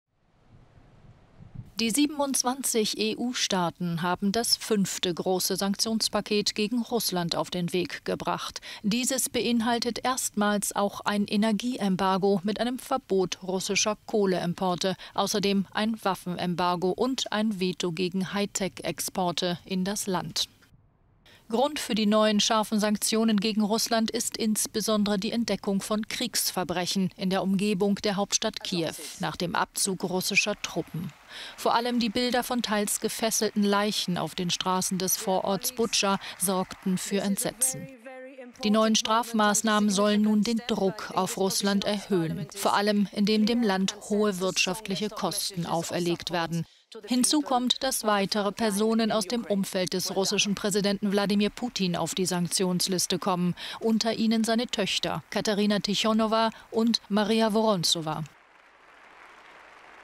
Gegen Kohle und Putins Töchter: 5. Sanktionspaket auf dem Weg
Die 27 EU-Staaten haben das fünfte große Sanktions-Paket gegen Russland auf den Weg gebracht. Dieses beinhaltet erstmals auch ein Energieembargo mit einem Verbot russischer Kohleimporte.